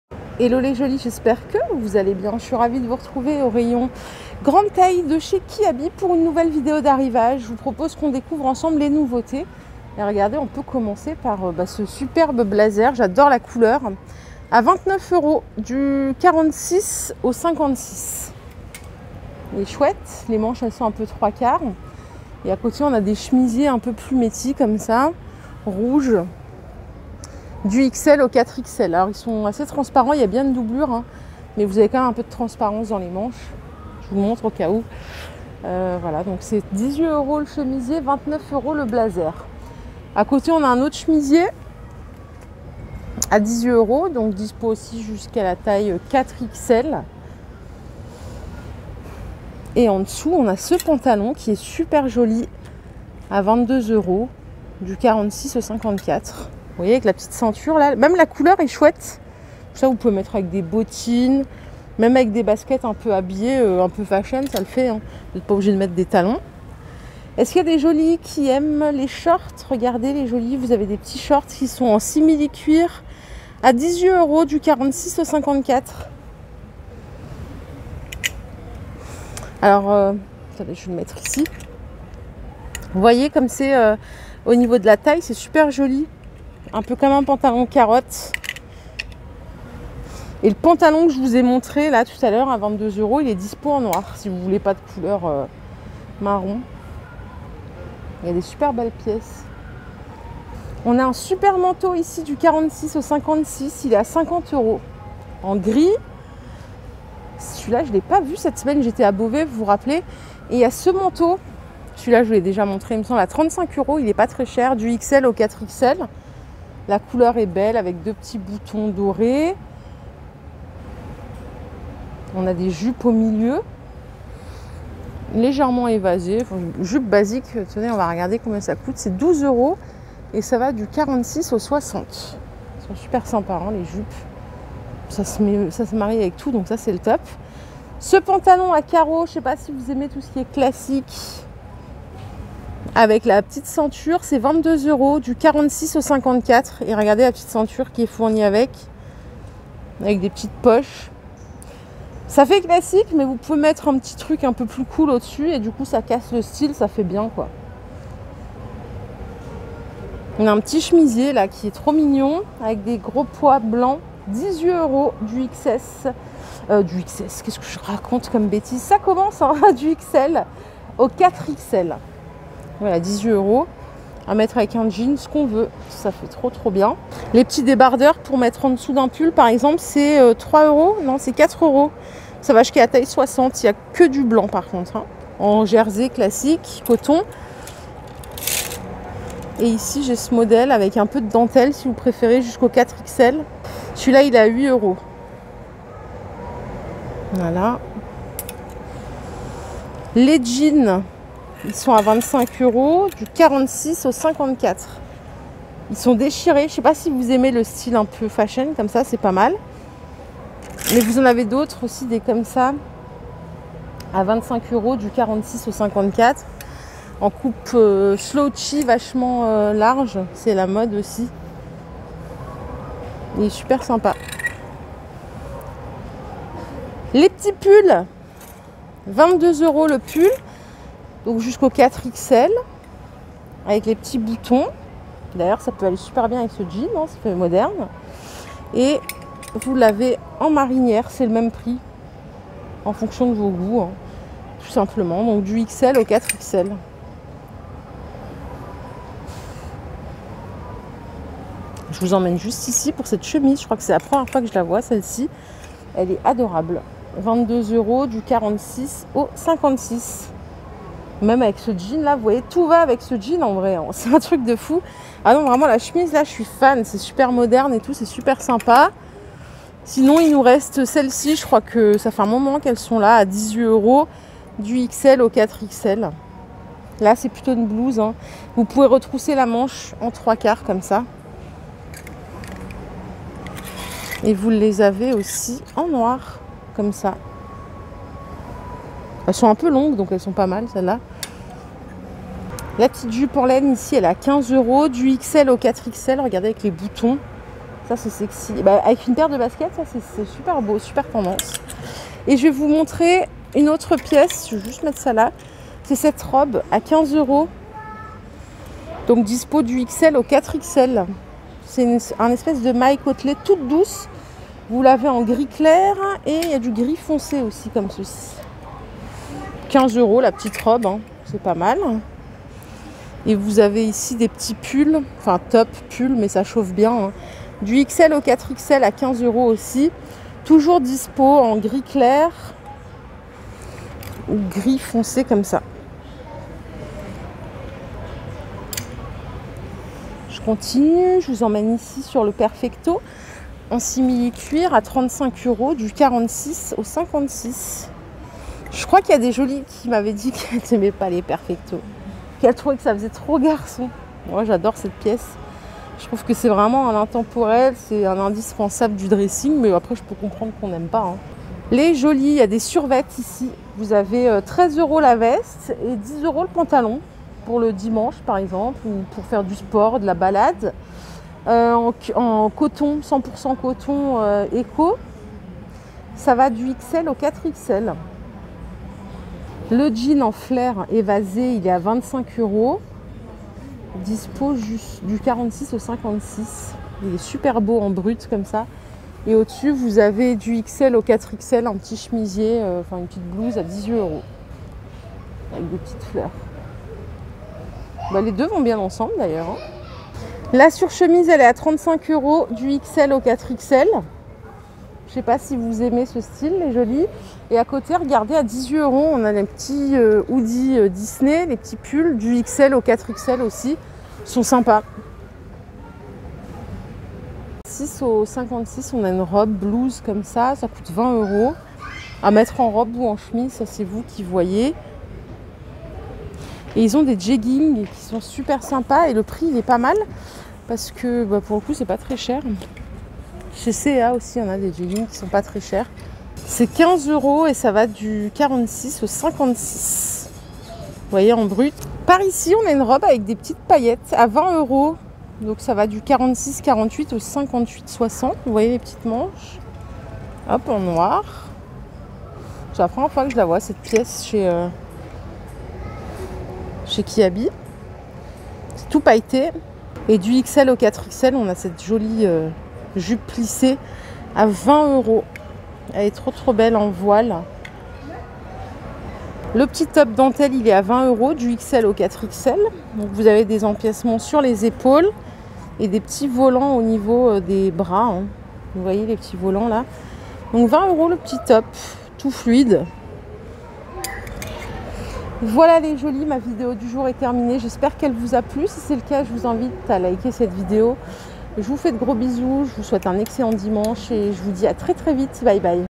🧥☃️KIABI MODE GRANDE TAILLE VÊTEMENTS NOUVELLE COLLECTION AUTOMNE HIVER 2022
Découvrez la nouvelle collection de vêtements mode grande taille automne hiver 2022 !

Superbes pièces pas chères : manteaux, pullover, robes, jupes pantalons !

Ne manquez pas les bons plans Kiabi 😜